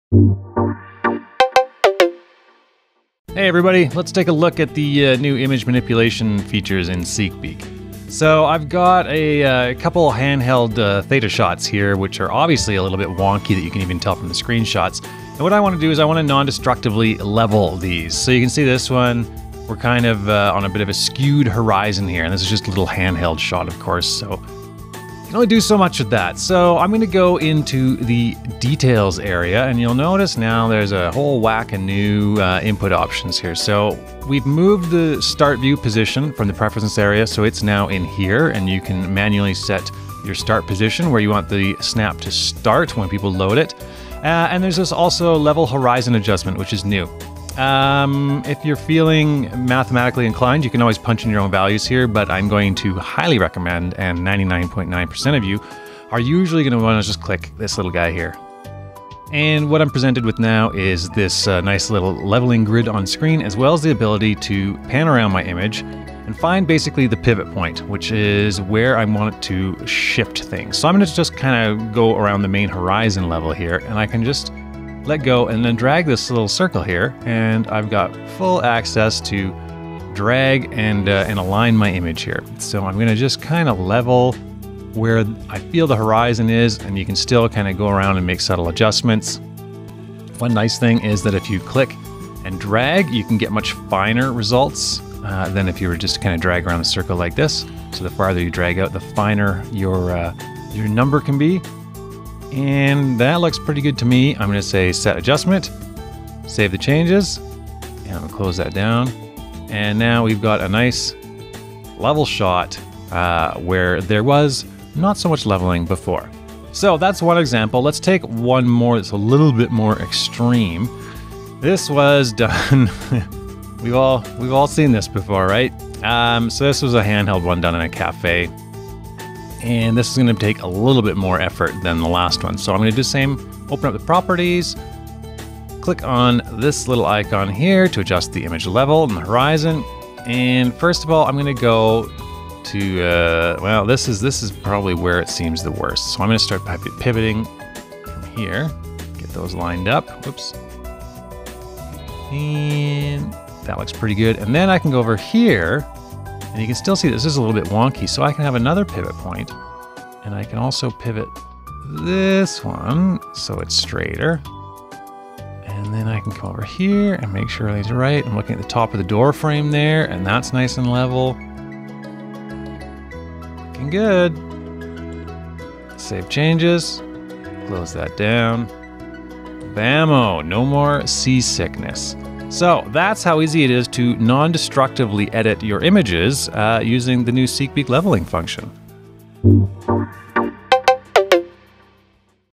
How To Straighten / Level the Horizon your 360º Images and Panoramas without Losing Quality
SeekBeak's new non destructive image leveling tools make it easy to fix any roll offsets, or horizon level issues with your 360º images and virtual tours.